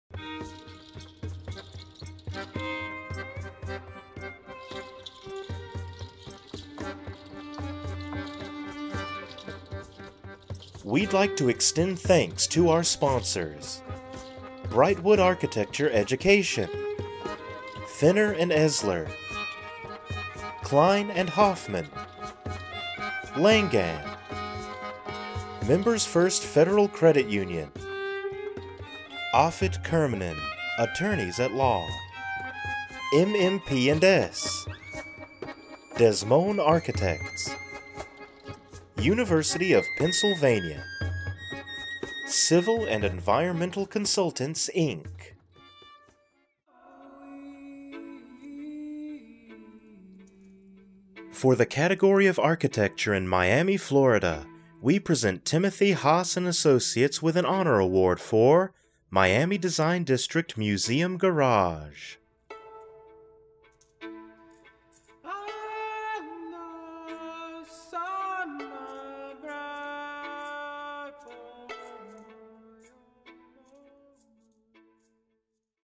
AIA PA Architectural Excellence Award | Miami Design District Museum Garage
Timothy Haahs & Associates, Inc. receives an Honor Award in the category Architecture for the Miami Design District Museum Garage located in Miami, FL